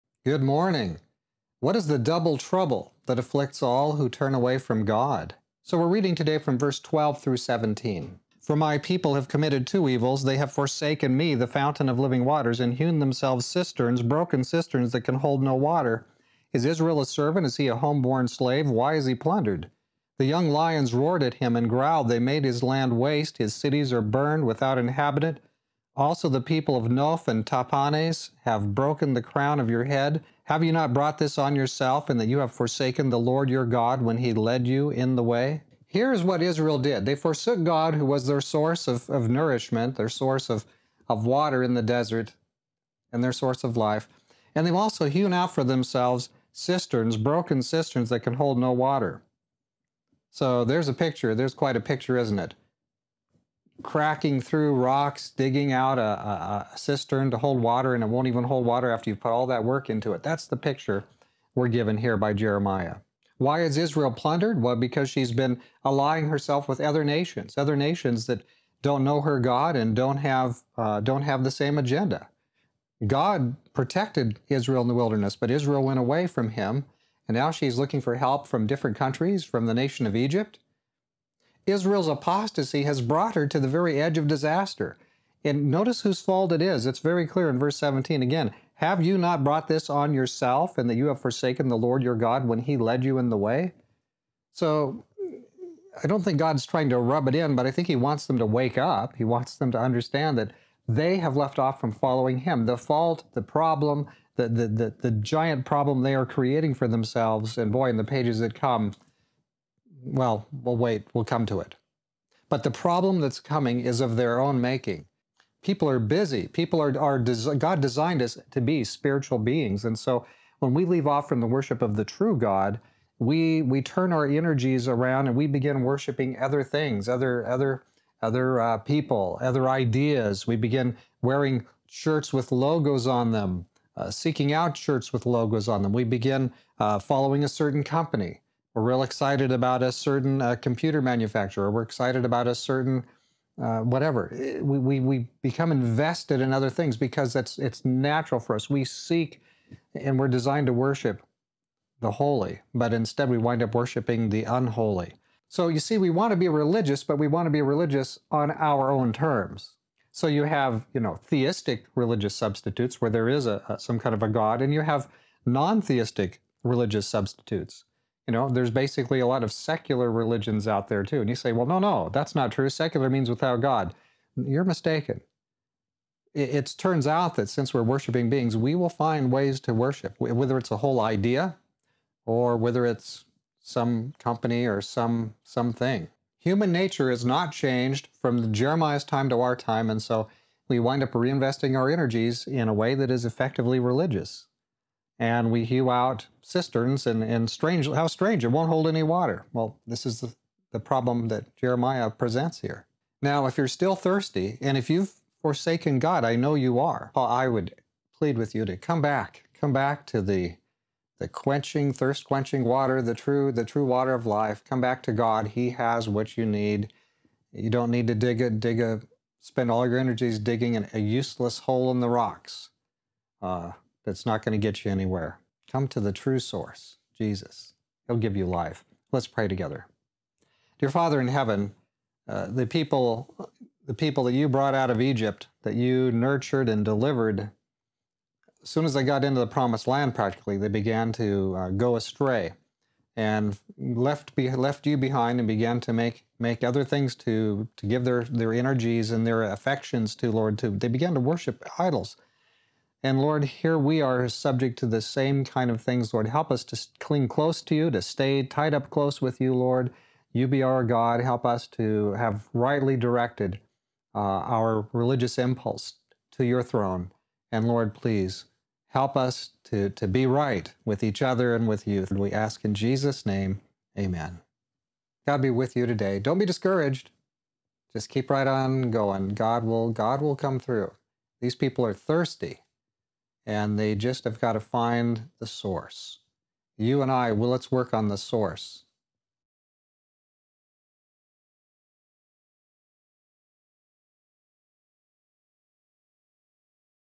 The Weeping Prophet Jeremiah 2:13-17 Broken Cisterns
What is the double trouble that afflicts all who turn away from God?

Parler.com: @SecondComing
Twitter.com: @SaviorAndMyKing